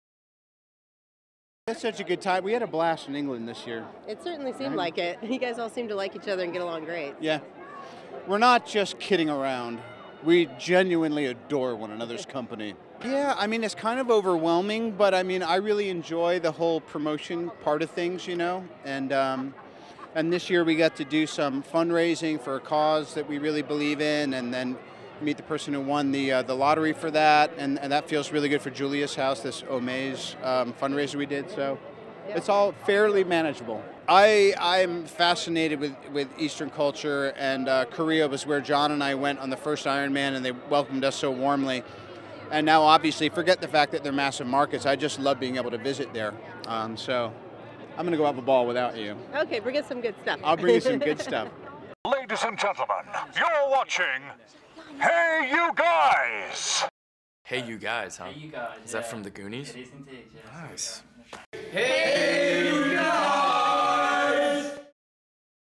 Robert Downey Jr. Interview - Avengers: Age of Ultron World Premiere
Robert Downey Jr. is interviewed at the World Premiere of the movie Avengers: Age of Ultron in Los Angeles.